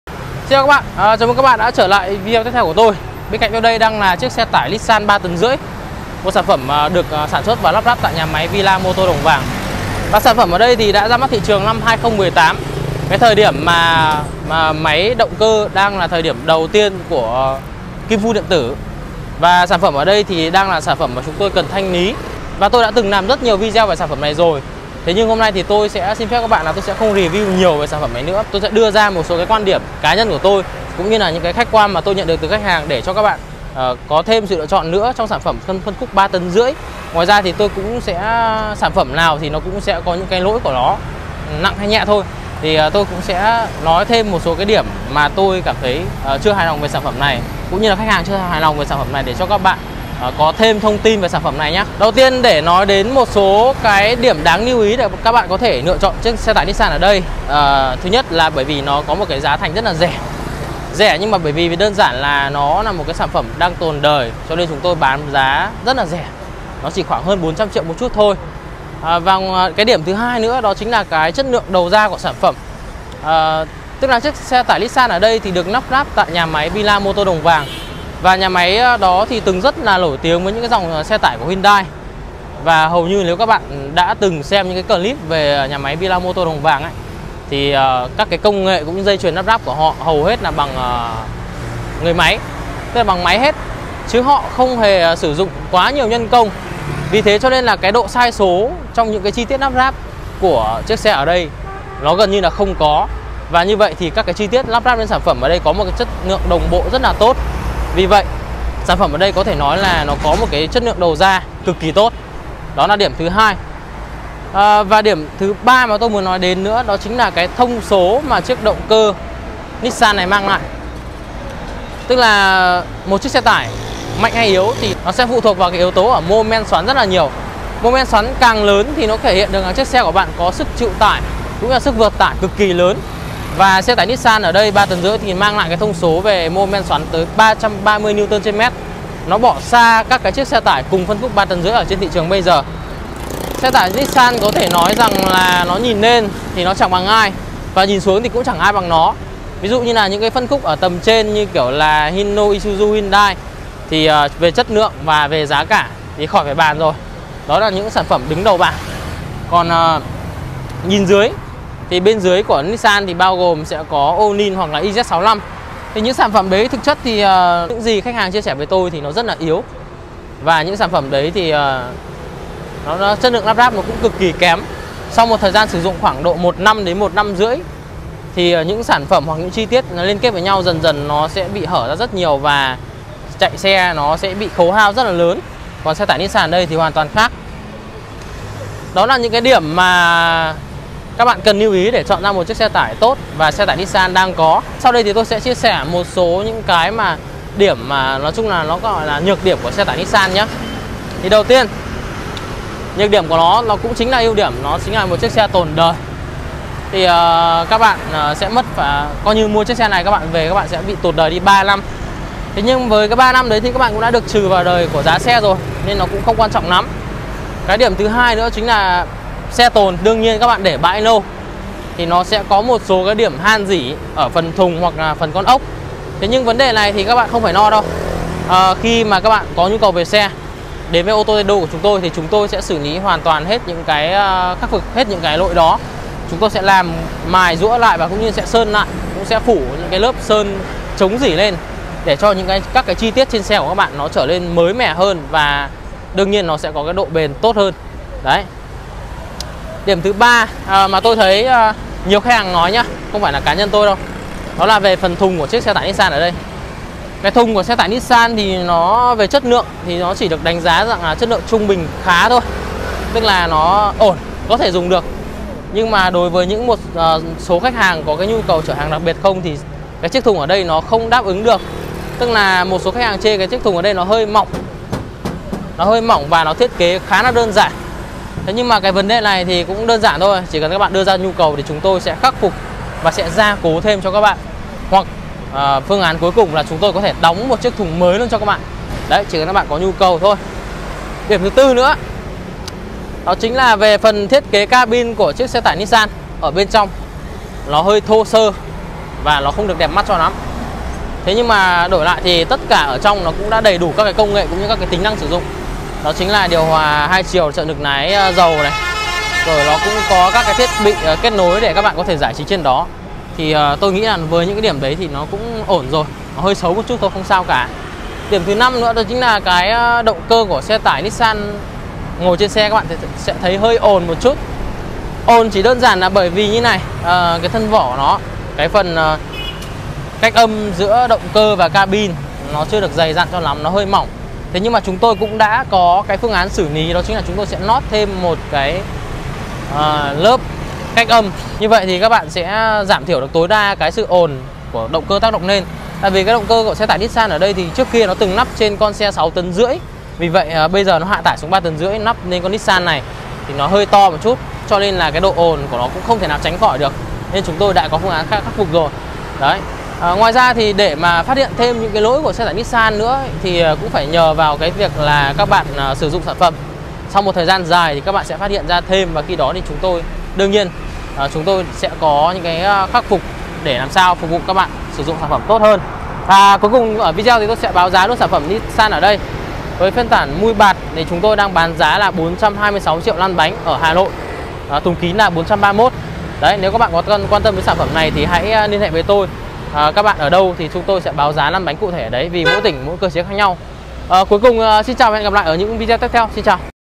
Những Ưu - Nhược Điểm Của Xe Tải Nissan 3.5 Tấn | Báo Giá Xe Tháng 7.
Xe tải Nissan 3.5 tấn ra mắt thị trường năm 2018. Là sản phẩm đang được thanh lý với giá thành cực kì tốt.
Ns350 đang được rất nhiều sự quan tâm của khách hàng. Đặc biệt là về những ưu nhược điểm cũng như là chất lượng của xe.
Trong video này tôi xin phép được đưa ra những
Ưu-nhược điểm của Ns350 đồng thời cũng báo giá sản phẩm đang hot này trong thời điểm tháng 7.